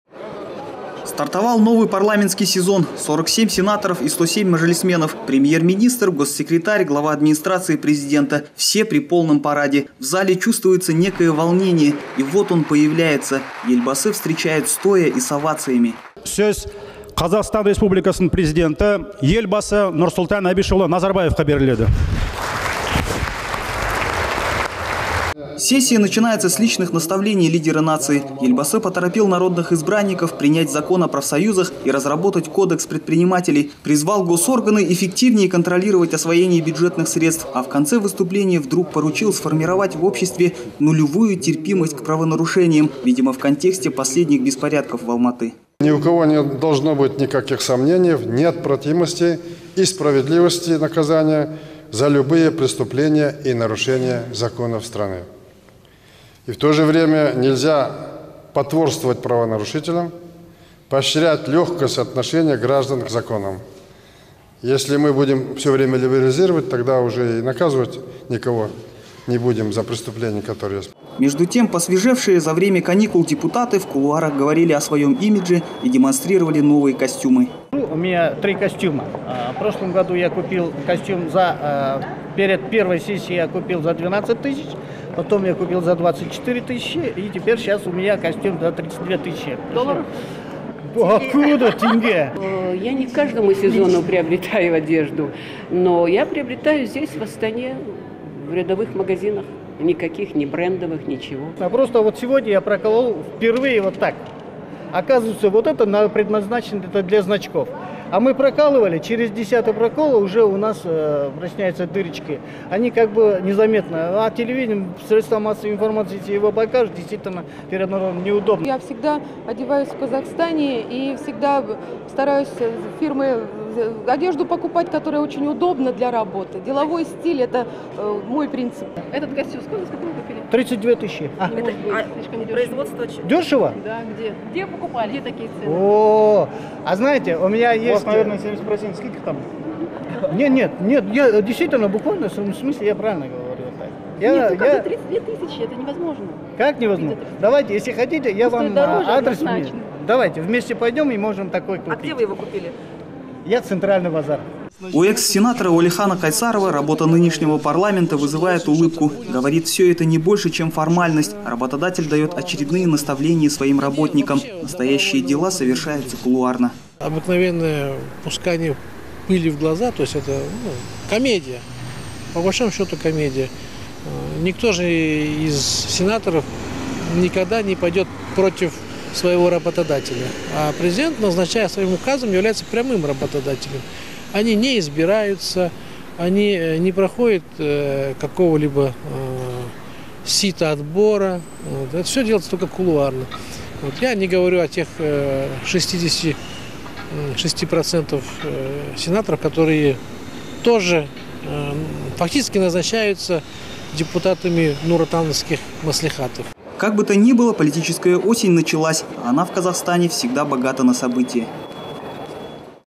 Назарбаев о "нулевой терпимости" к преступлениям и депутаты о костюмах / 1612
Елбасы торопил народных избранников: принять закон "О профсоюзах" и разработать Кодекс предпринимателей. Призвал госорганы эффективнее контролировать освоение бюджетных средств. А в конце выступления вдруг поручил сформировать в обществе «нулевую терпимость к правонарушениям», видимо в контексте последних беспорядков в Алматы. Посвежевшие за время каникул депутаты в кулуарах говорили о своем имидже и демонстрировали новые костюмы.